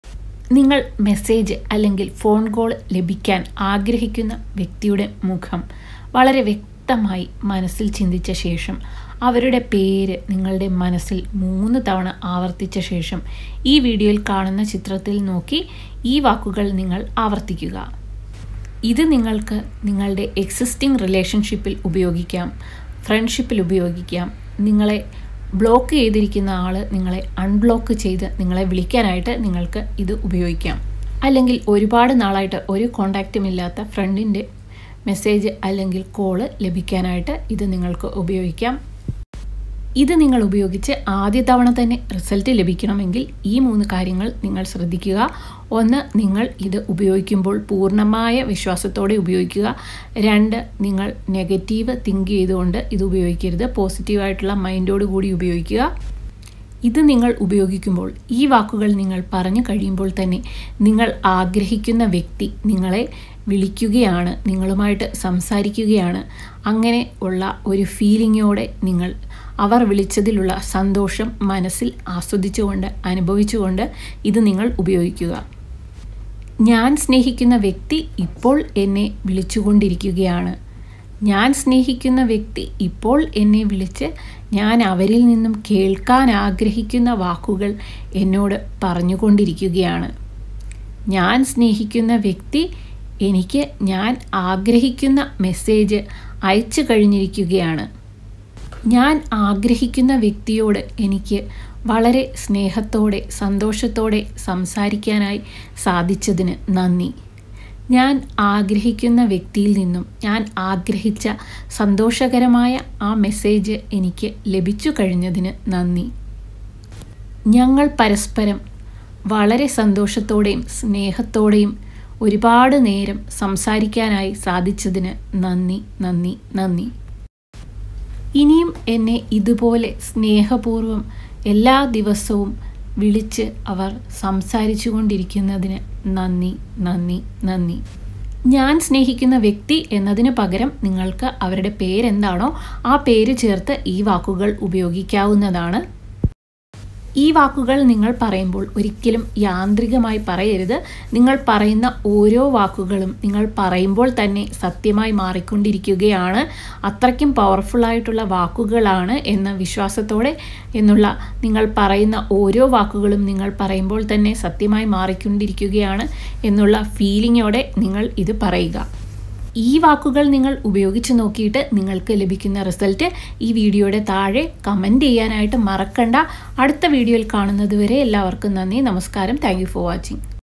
Do This If they Are Not Calling You- Law Of Attraction Malayalam
In today’s video we are sharing with you a very simple and powerful technique to get a phone call or message from your specific person. This can be used to get calls in an existing relationship or friendship or from an ex or from someone who blocked you.
How to use this?
How powerful is this?
How long to use this?
How fast you will get results?